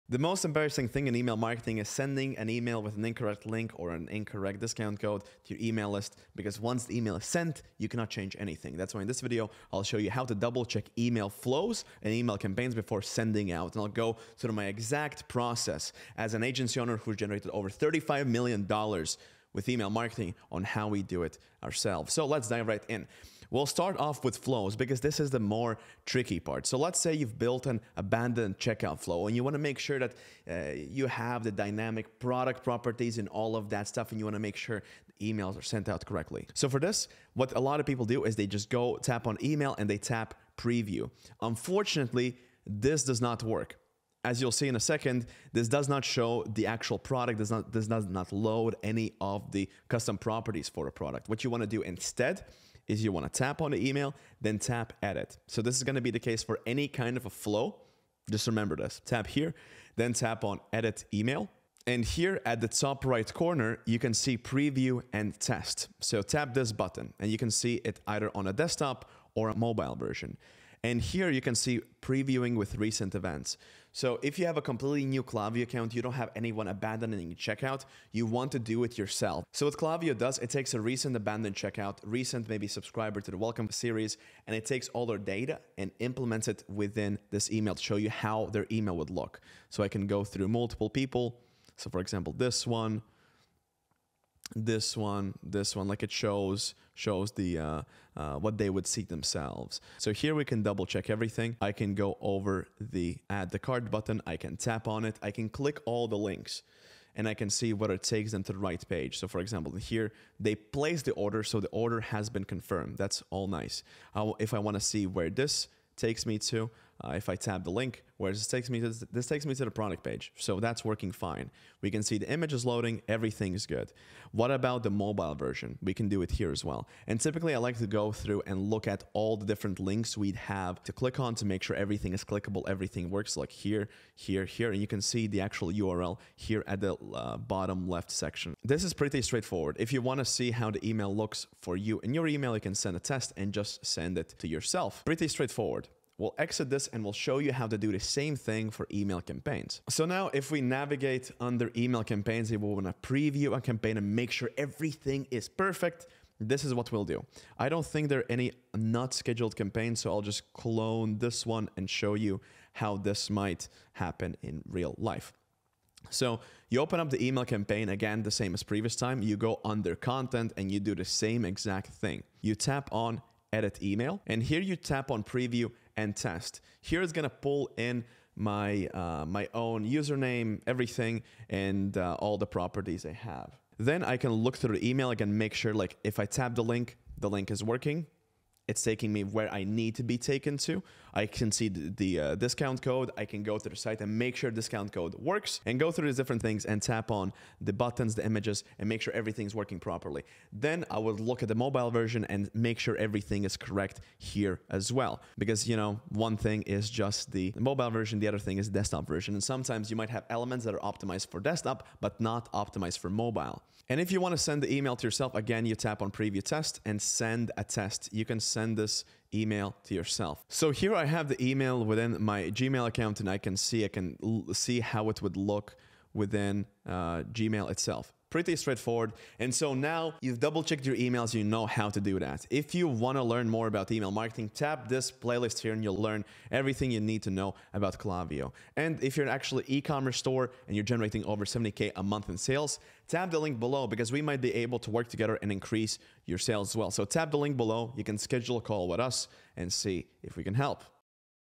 How To Send a Test Email Klaviyo 2023
I'll show you how to send a test email in Klaviyo which allows you to test Klaviyo flows and campaigns. So you can avoid sending emails with broken links, or the wrong text.

We'll Increase Email Sales by 30% Or You a refund

TIMESTAMPS:
00:00 The Problem
00:23 How To Test Emails In Klaviyo Flows
02:28 How To Test Klaviyo Email Campaigns